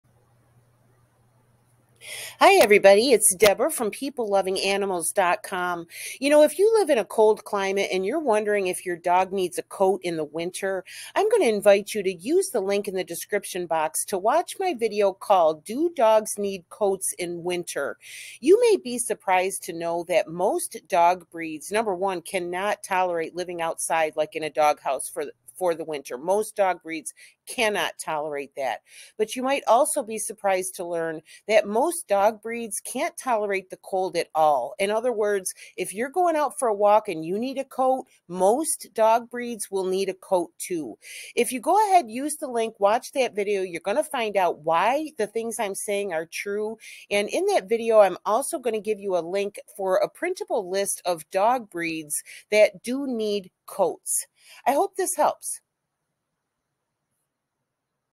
Do Dogs Need Coats in Winter? dogshorts | When is it Too Cold for Dogs?
If it's so cold that you feel you need to bundle yourself up to go outside, you should also consider whether your dog needs protection from the cold as well.  Do dogs need coats in winter? Let's discuss which breeds do (you may be surprised), reasons why your dog might need to wear a coat, how to know if your dog is going to be too cold, and which type of dog coat to buy. Debra :)

For more information about keeping your dog safe in cold weather, refer to the resources below, and watch these videos:

Information Sources:
American Kennel Club: Does My Dog Need a Winter Coat?
OneHowTo.com: How to Know if My Dog is Cold
TopDogTips.com: 8 Telltale Signs Your Dog is Cold